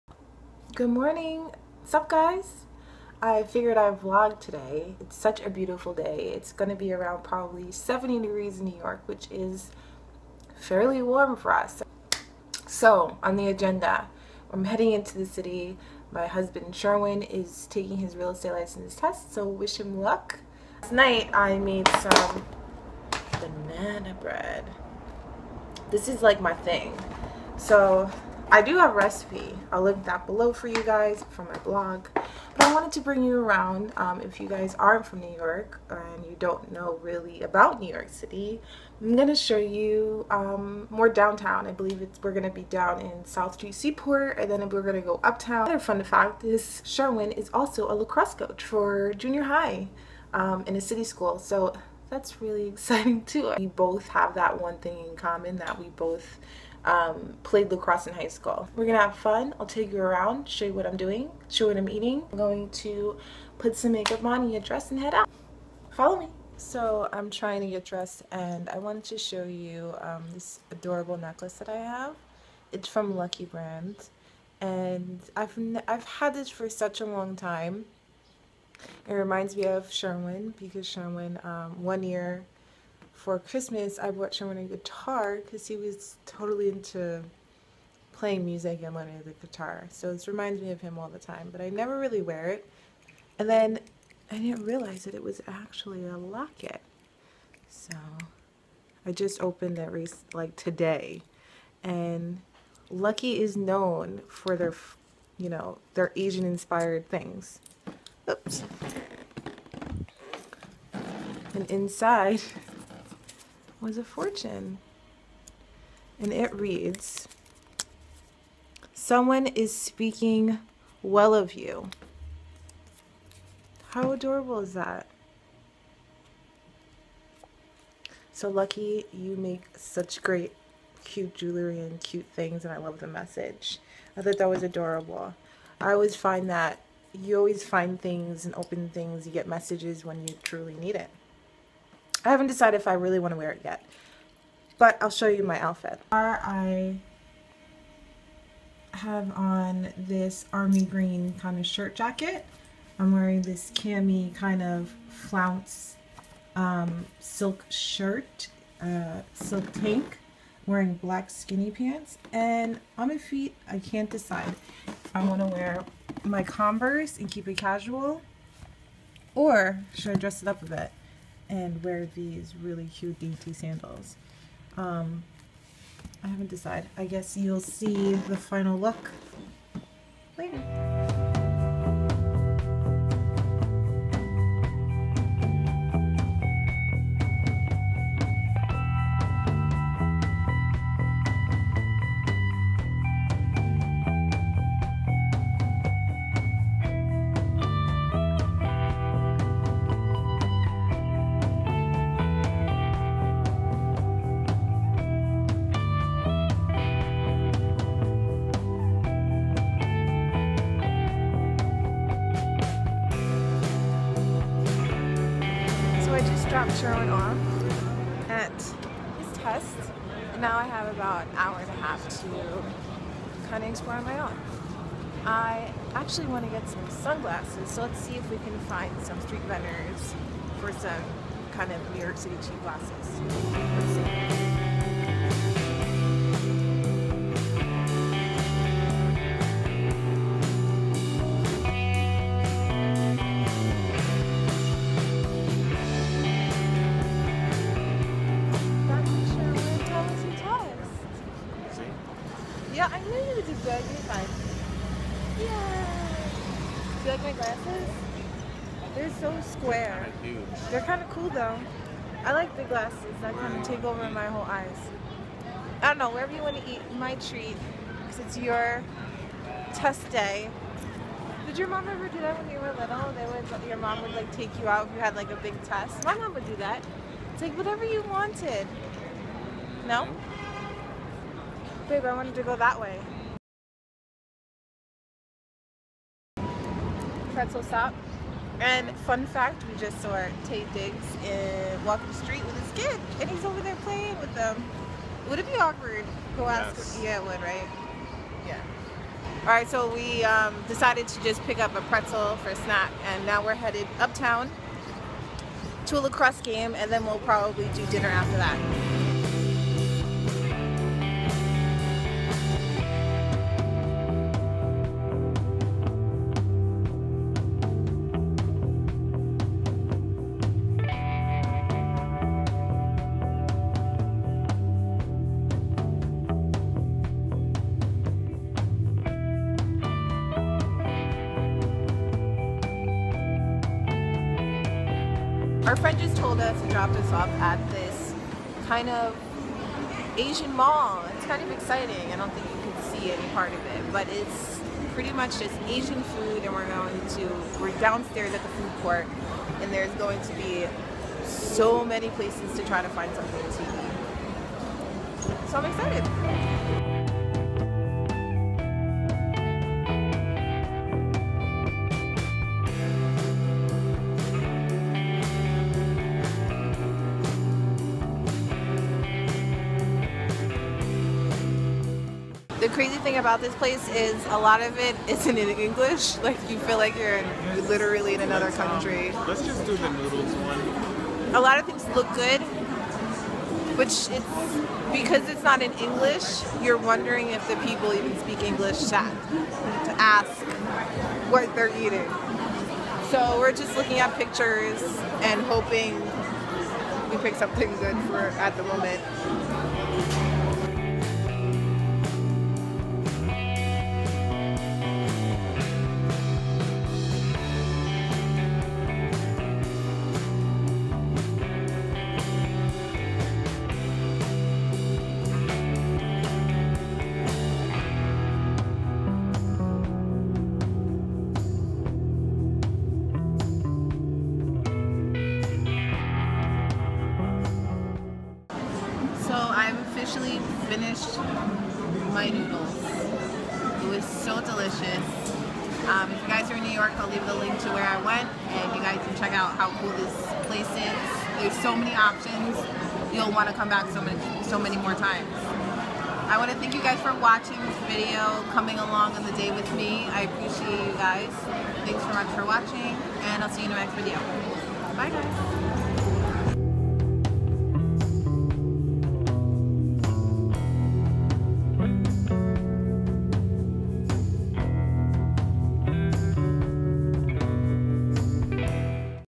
Vlog 2: New Sunglasses | *Spoiler - Bagel Twerking in NYC
I thought I'd share my day with you. In this NYC vlog, I head downtown, find fun cheap sunglasses, a bagel twerking, walk uptown to Randall's Island and exploring an asian mall in the heart of Flushing. If you enjoy these vlogs give this video I thumbs up so I know to film more!